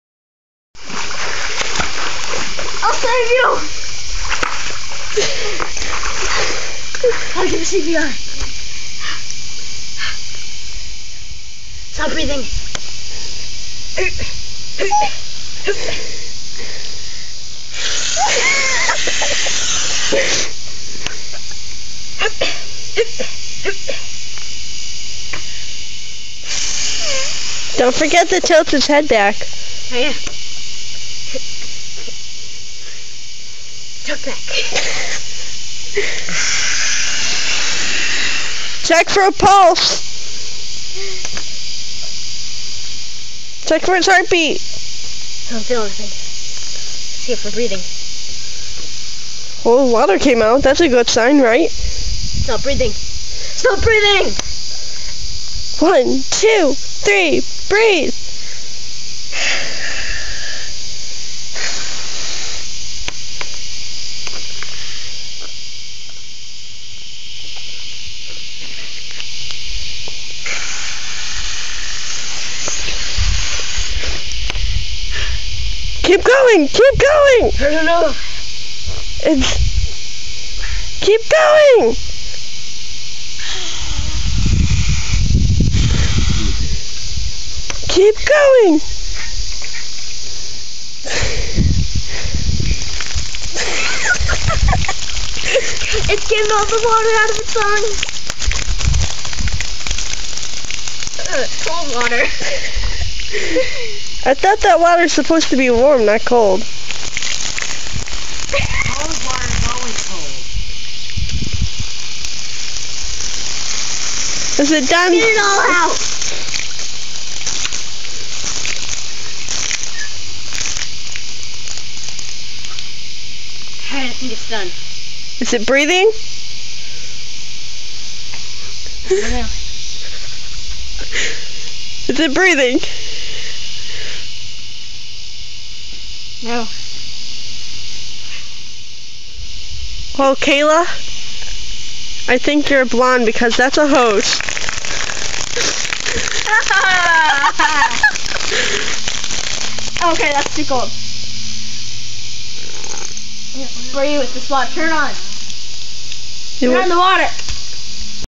Girl gives CPR to a hose
Me giving,CPR to my,hose to be funny.dont mind my,grandma in the,background,saying,all,hose water is,cold. She didnt know we were video taping.